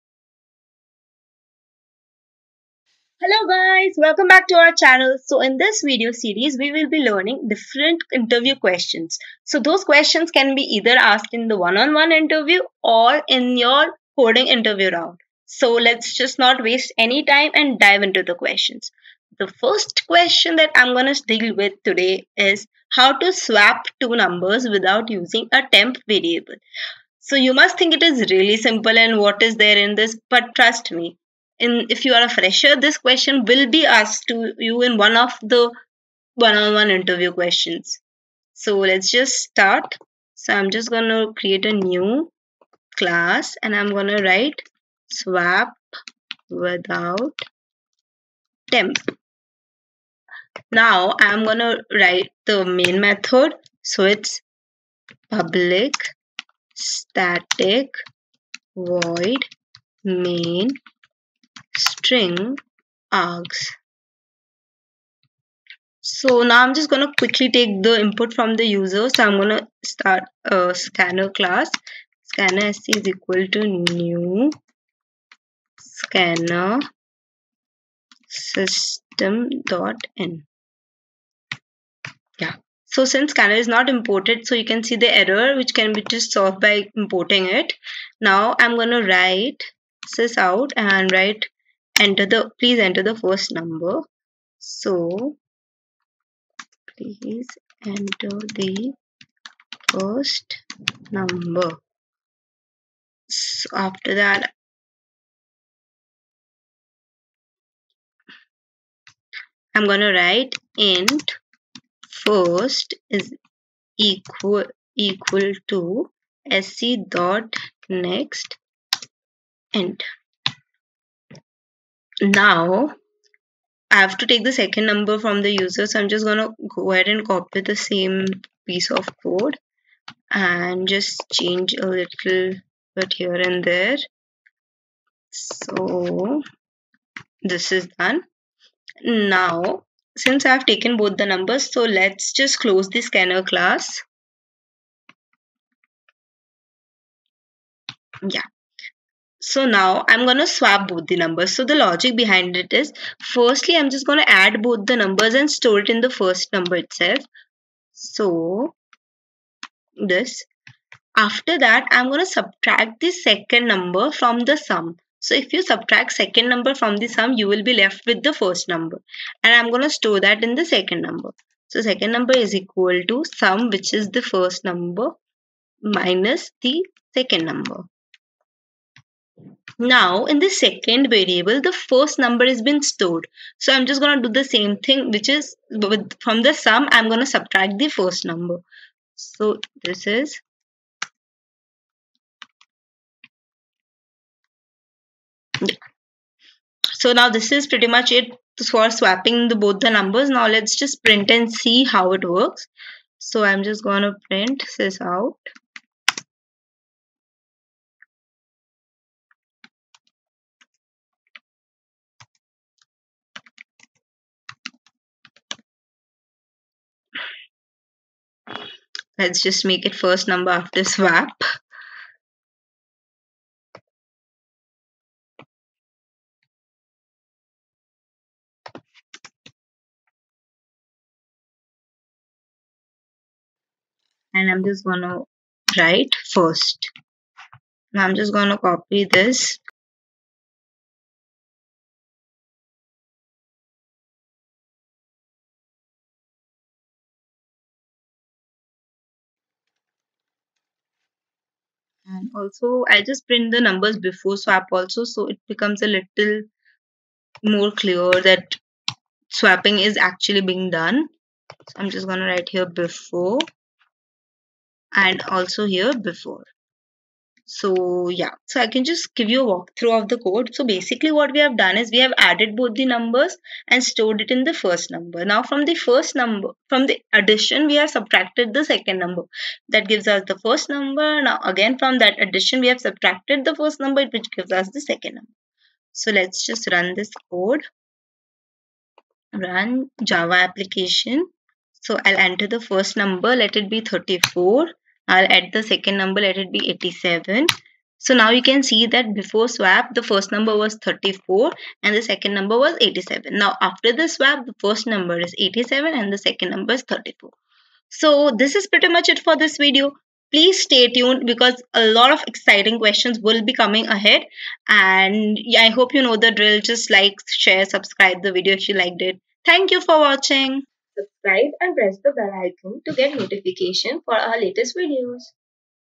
Interview Question : Swap without using Temporary(Third) variable.
This is the first video of our Coding Interview Question Series wherein we will be learning either Coding Round Question or Technical Interview Question.

Problem Statement: Swap two numbers without using a temporary or third variable.